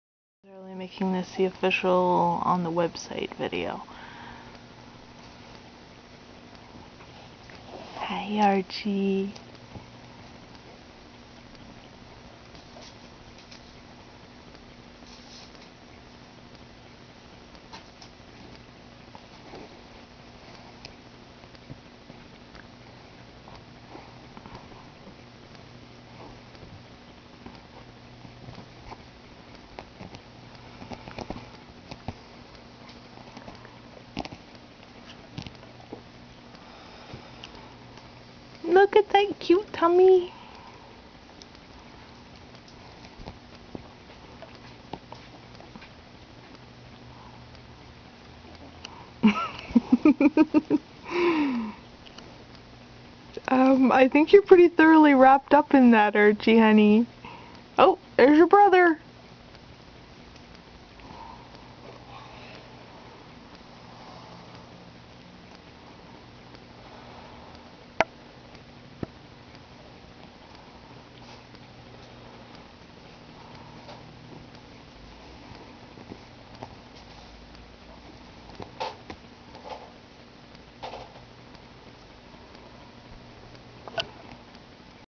Archie's Evening of Wrestling With String Theory
This is my sweet foster-kitten Archie -- originally the smallest and most ill when they came here, and had to be coaxed into eating.  He's no longer the smallest, is wonderfully strong and healthy, and is a very loving little charmer.  Playful, too!

Archie needs a forever home.  You can contact the Kingston Humane Society his shelter ID is 09-985B.